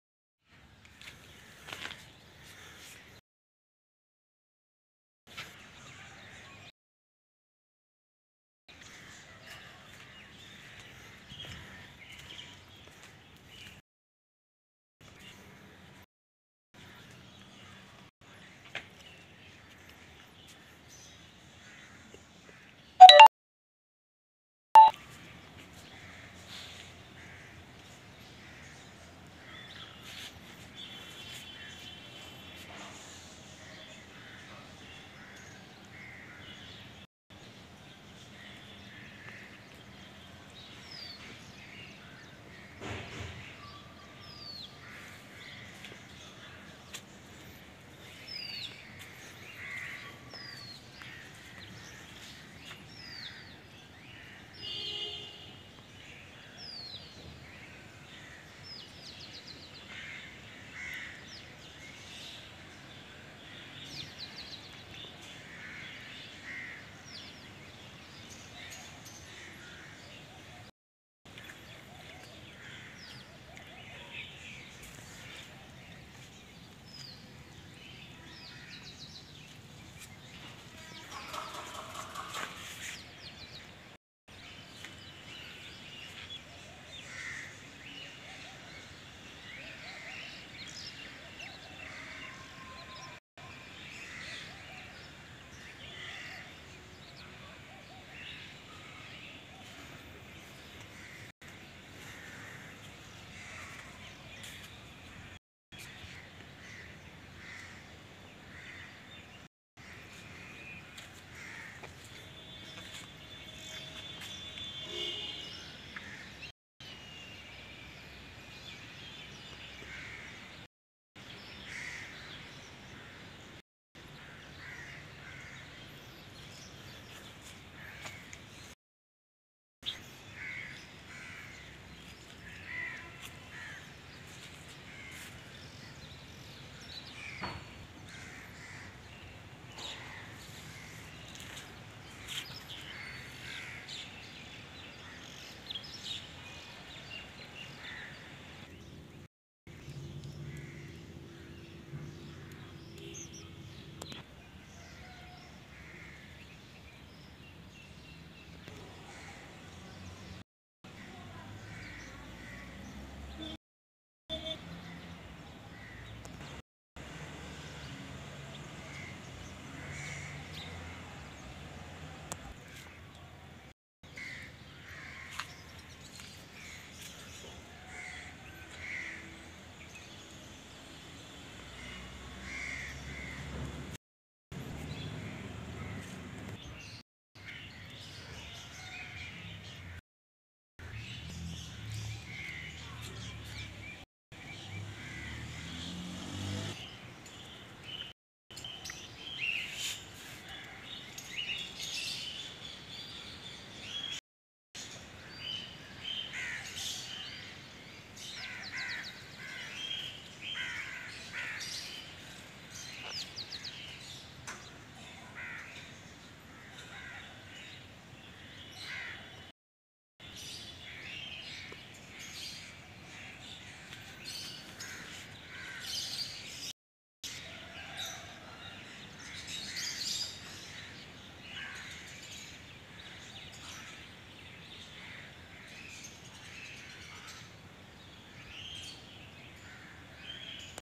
Rooftop garden update 11/09/2023
I just made this video in the morning after I finished watering my plants, I know my morning glorry gona be Mad in next few weeks, I will share another update again to show how the morning glory flowers are making my Rooftop so beautiful ❤️.